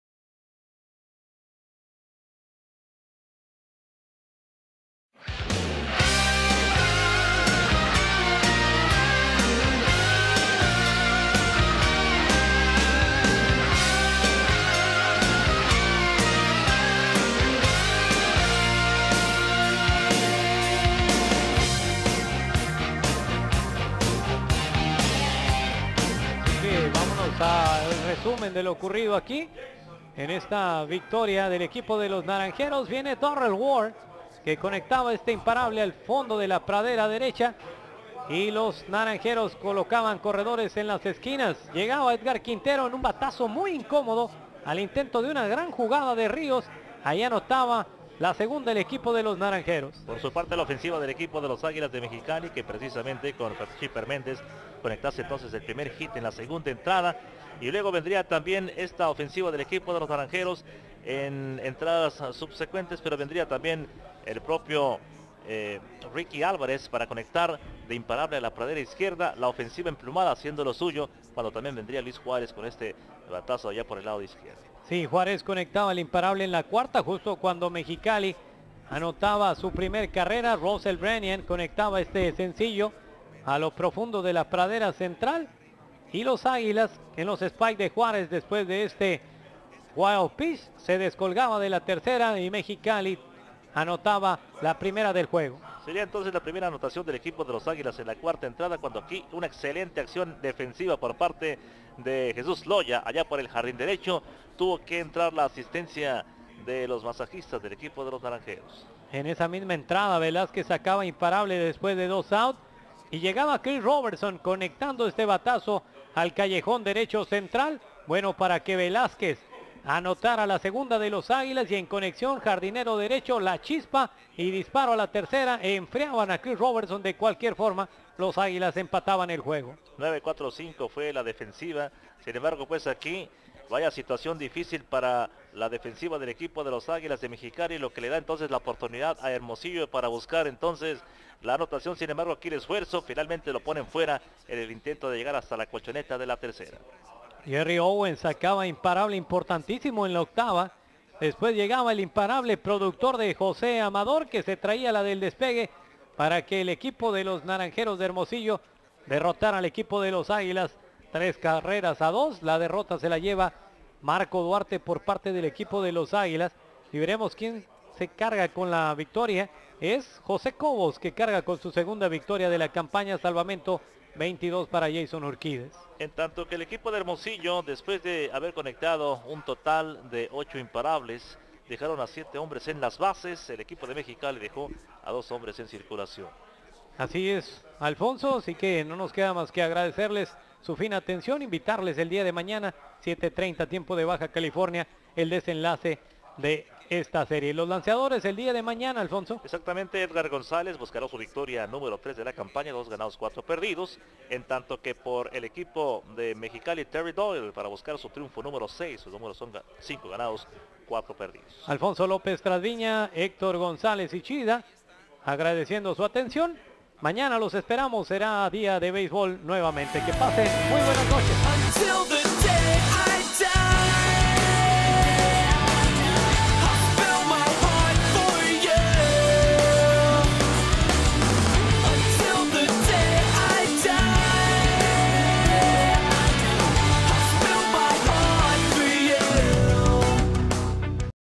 Naranjeros - Águilas, 26 de diciembre 2013.
Naranjeros de Hermosillo 3-2 Águilas de Mexicali.
Estadio Casas GEO.
Mexicali, Baja California, México.